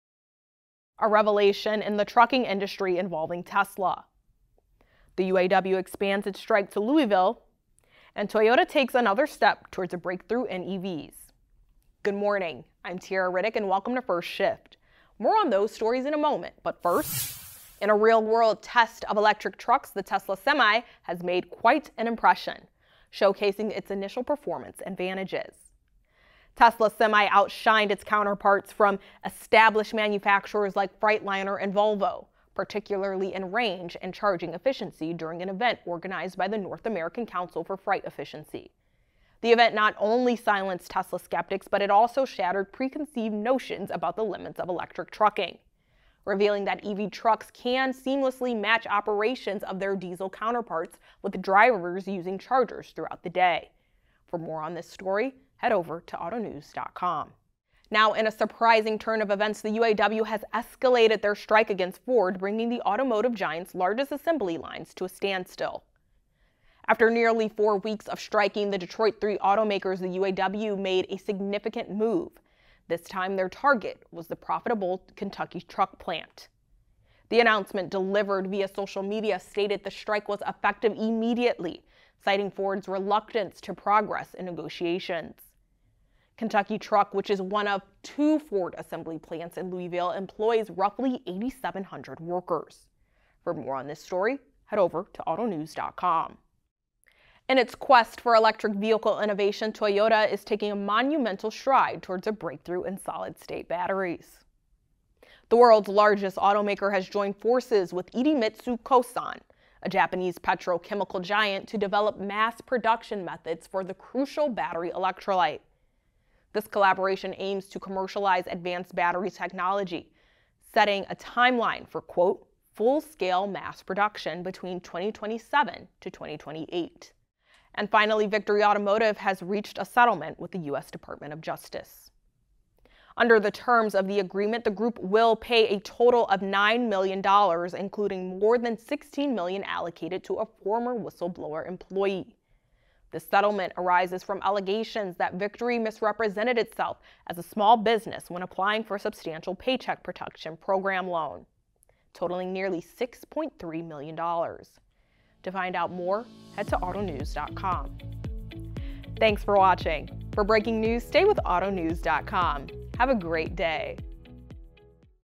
FIRST SHIFT - 10/13/2023: A real-life look at Tesla Semi
Tesla Semi real-life test | UAW strikes Ford Kentucky | Toyota's battery partnership